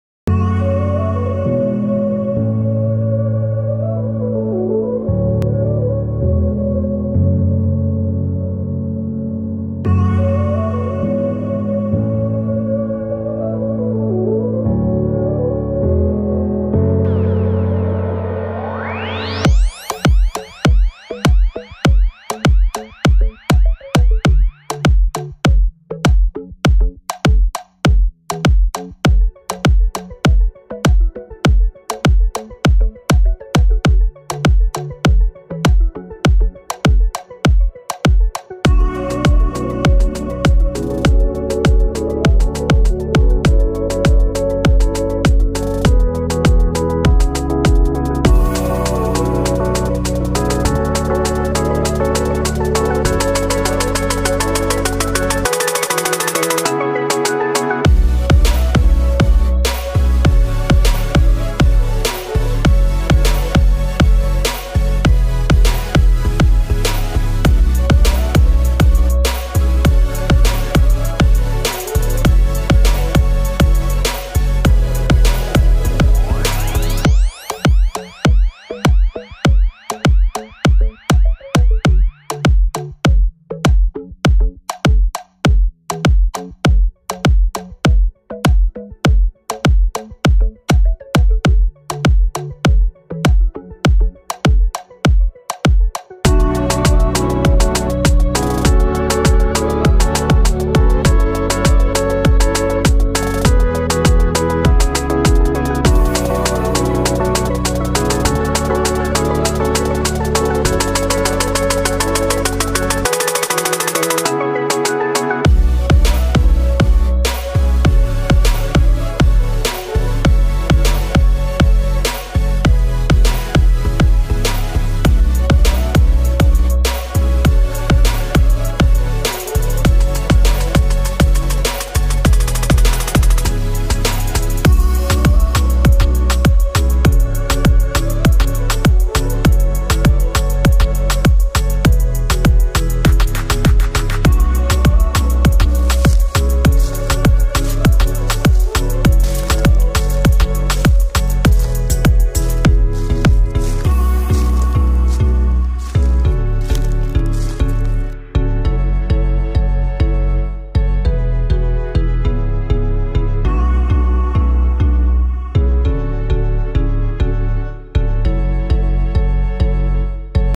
Nature love 🌱🌳🌴🌹landscape, waterfall, relaxing music // good sleep// rain// animal//horse//cheetah
Hey Mood Dreamers,

Relaxing Nature love scene with music to give you pleasant and positive vibes to make you happy and cheerful. 🌸🌸
It will relax your mind and fill your body with active and positivity 🍀